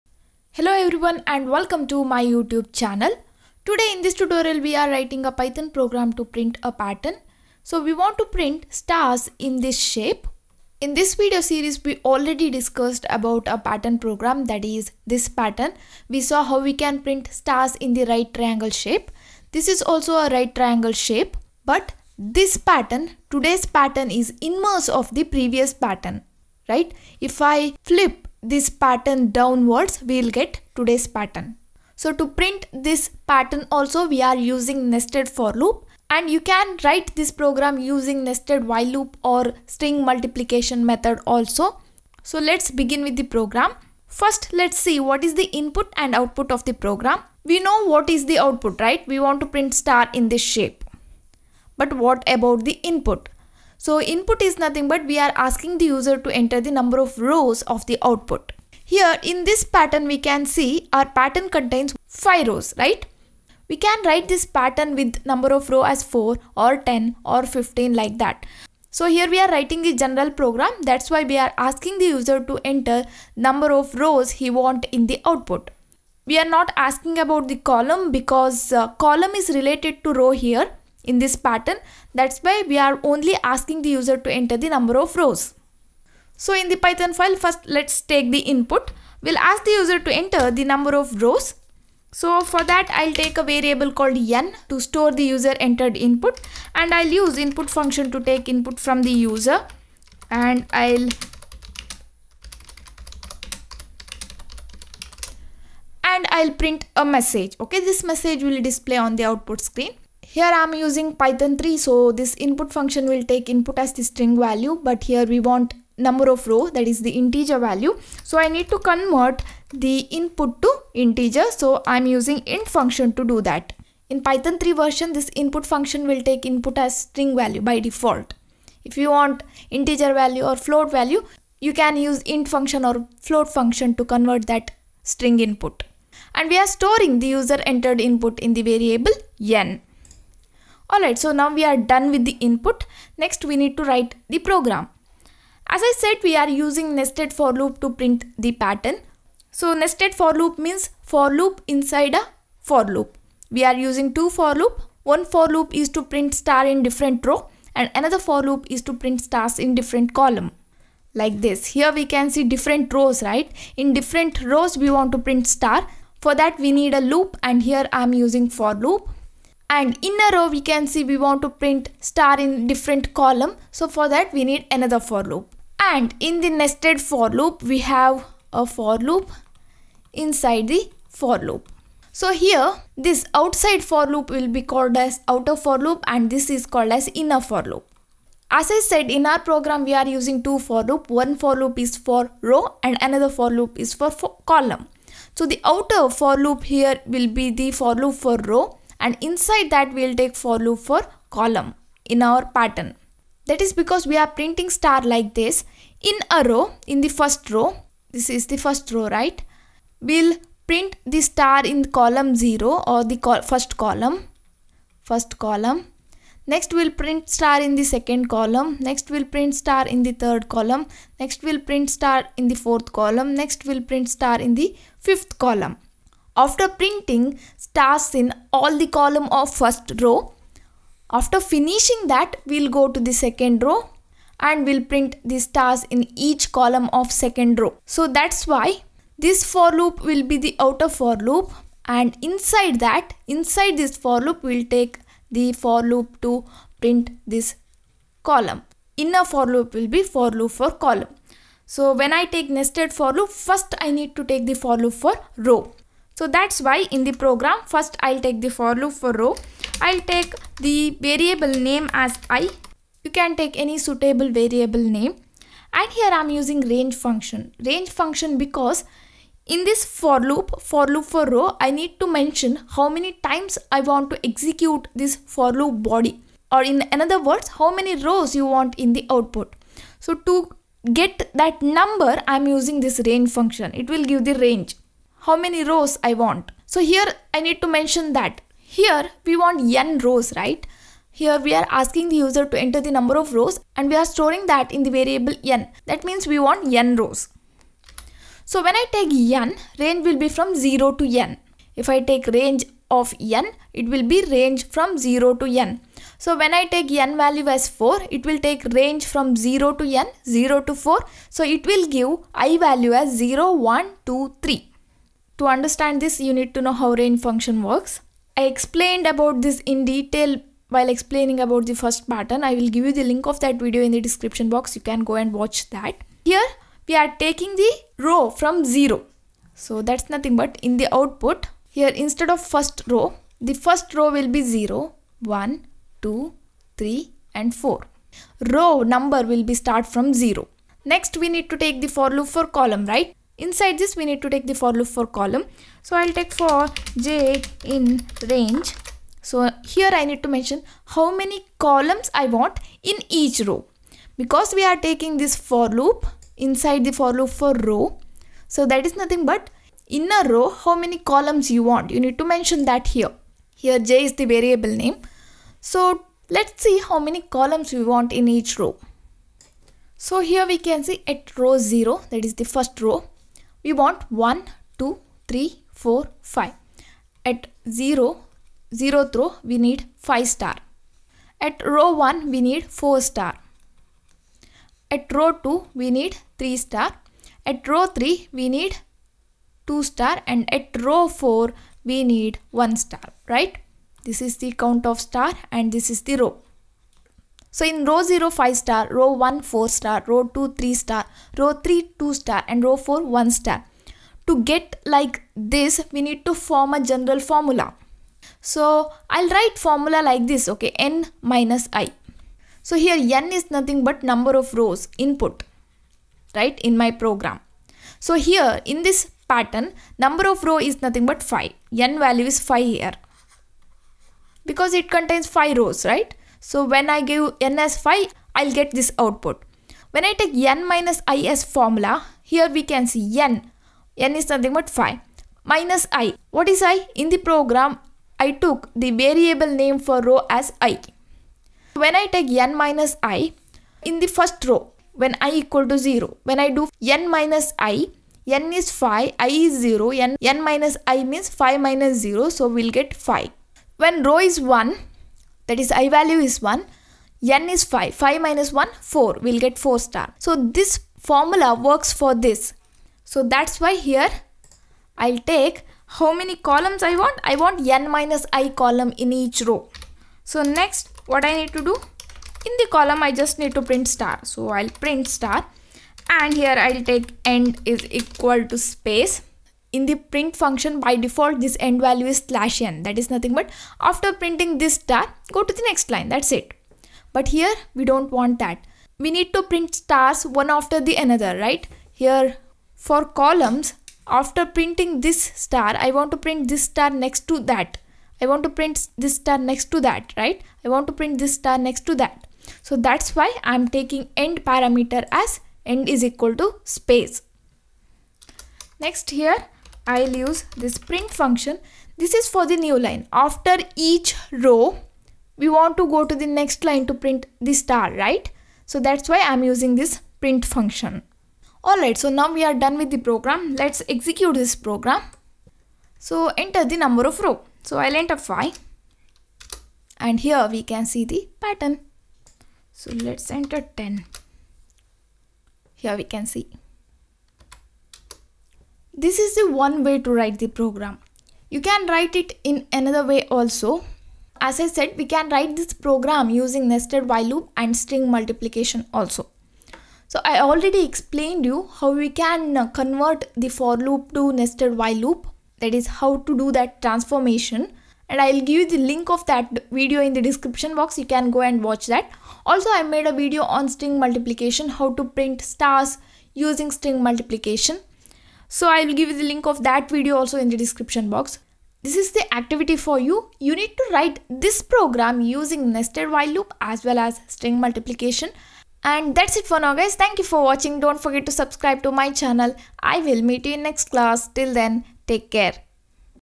Printing Stars "*" in Inverted Right Triangle Shape | Python Pattern Program
In this python programs video tutorial you will learn to print stars in right triangle shape in detail.

We used Nested for loops to write this program.